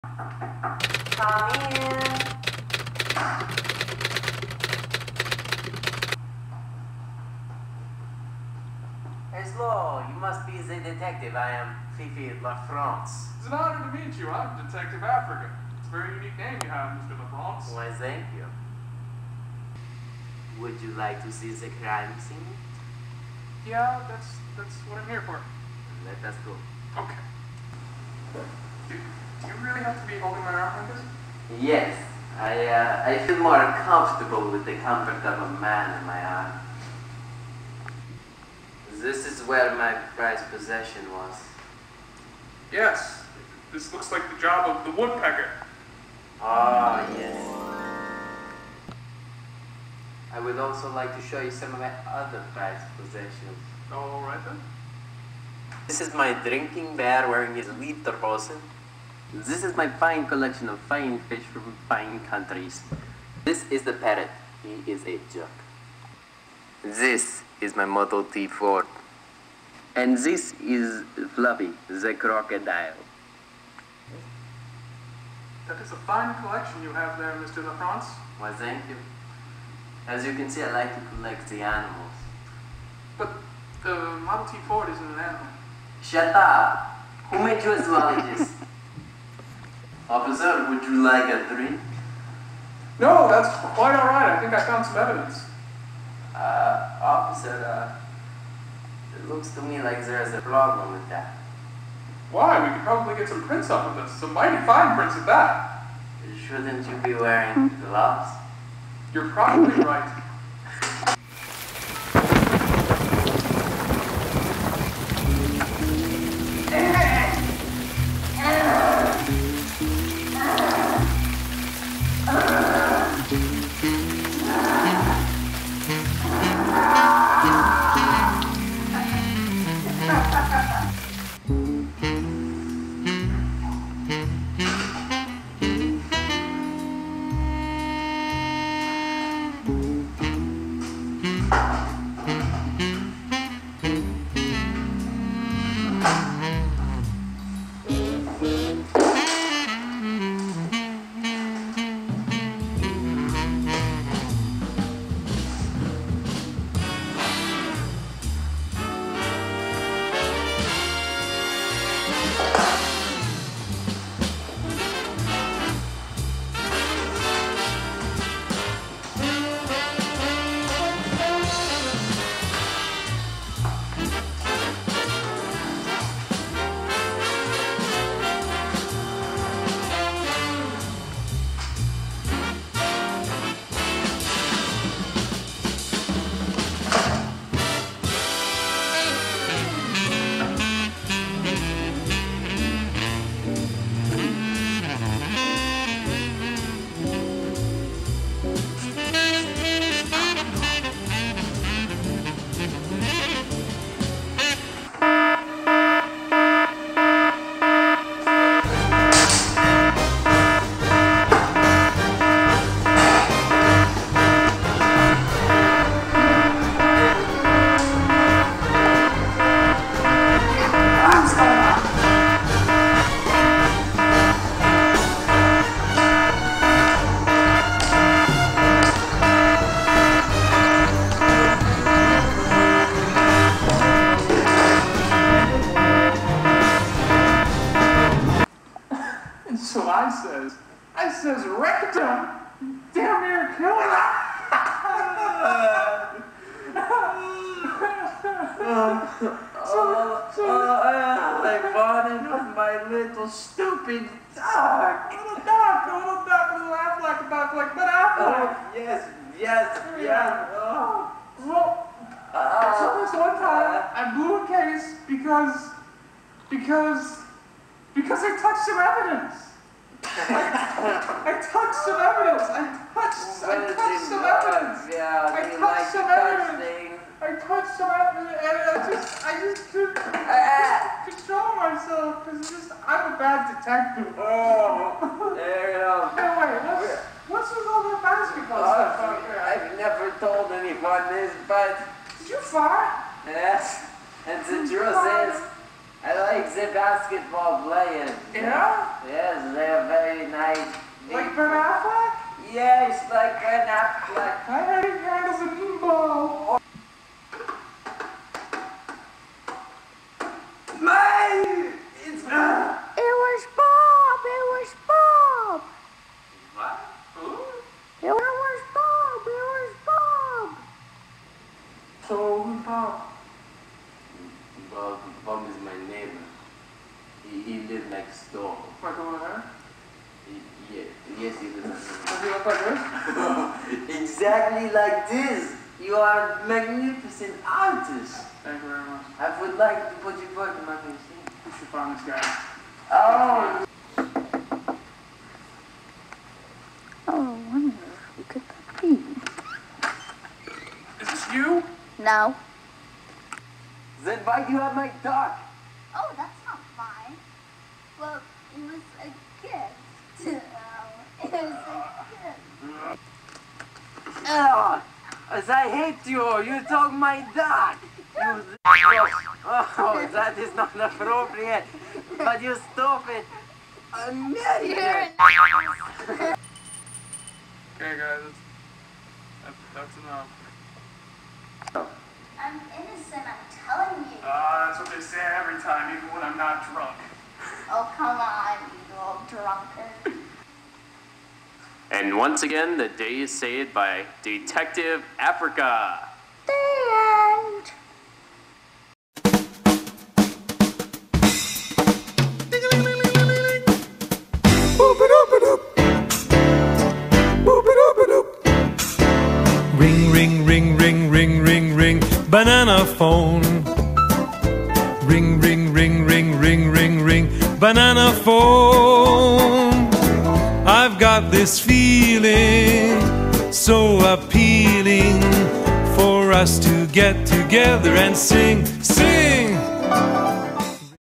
Detective Africa Case File #001- The Wild Woodpecker
From the mind of an eleven year old to the inside of your eyelids comes the most ferocious detective this side of the Saraha desert. IT'S DETECTIVE AFRICA!